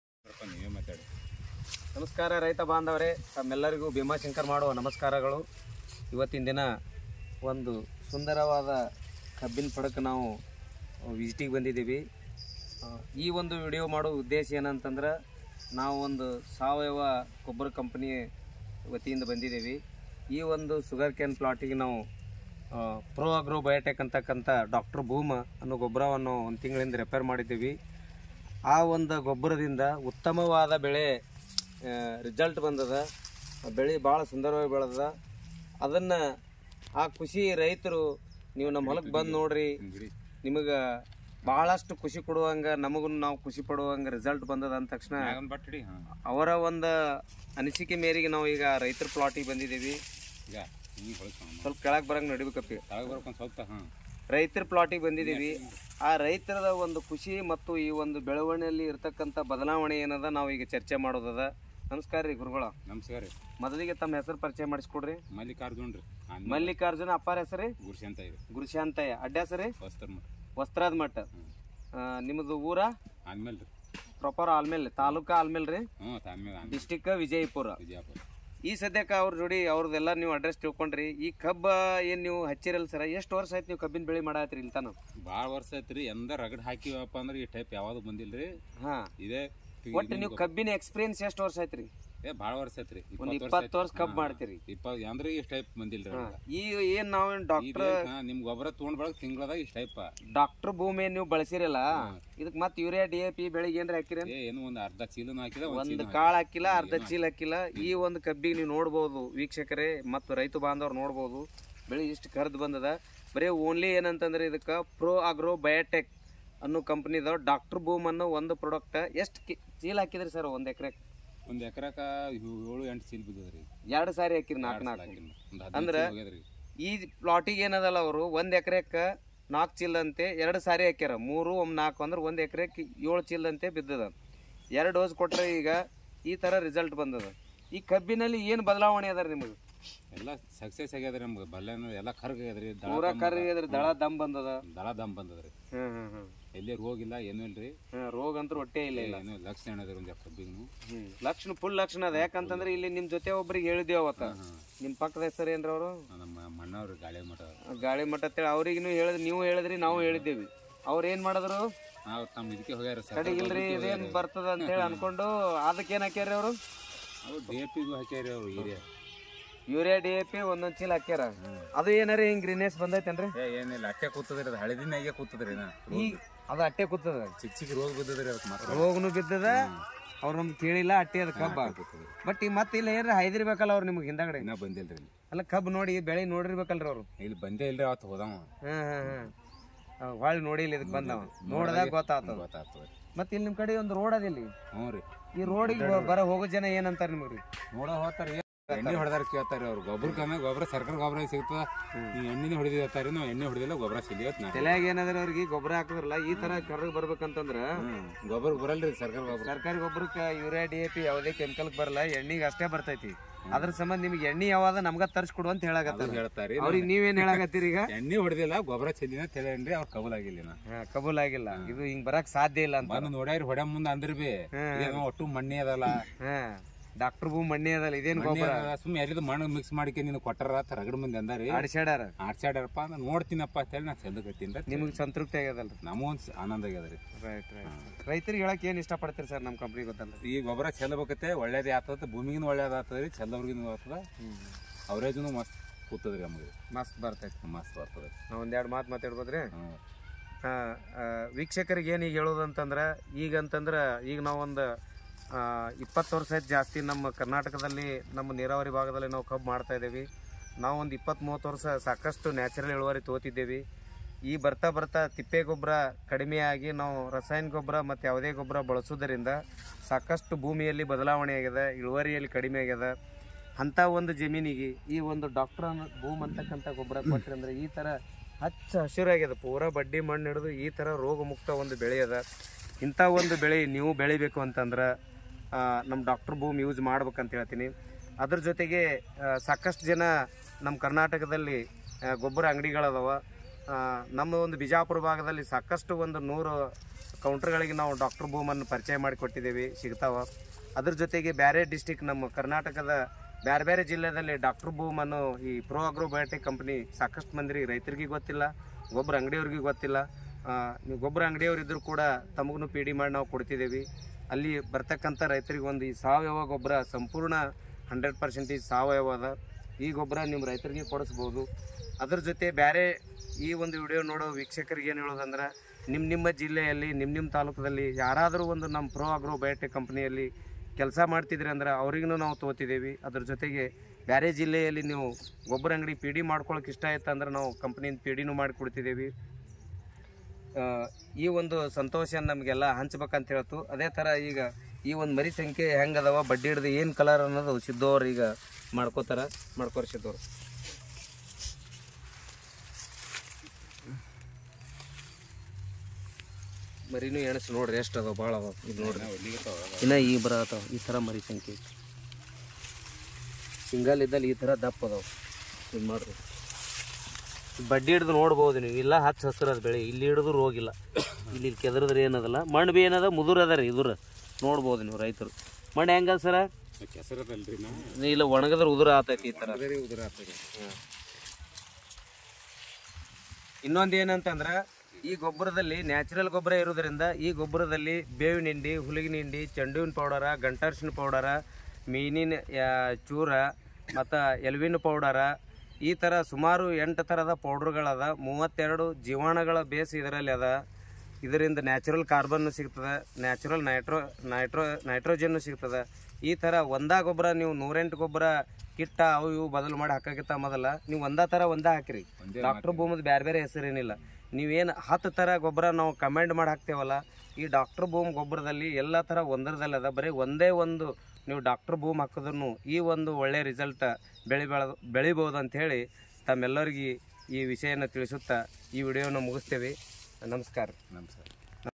Dr Boom ಕಬ್ಬಿಗೆ ಒಂದು ಉತ್ತಮವಾದ ಗೊಬ್ಬರ
ಡಾಕ್ಟರ್ ಬೂಮ್ ಸಾವಯವ ಗೊಬ್ಬರ ಎಲ್ಲ ಬೆಳೆಗಾಗಿ ಉತ್ತಮವಾಗಿದೆ